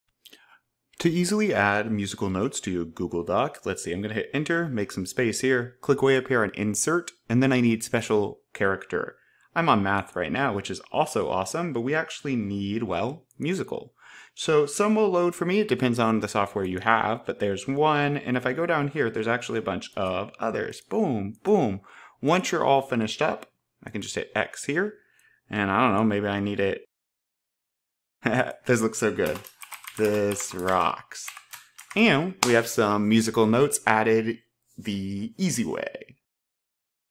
Add Music Notes to Google Docs the Easy Way
Google Docs is a power tool with packed with features. In this tutorial I walk you through step-by-step how to utilize Google Docs to the fullest. A playlist of Google Docs tutorials is linked below.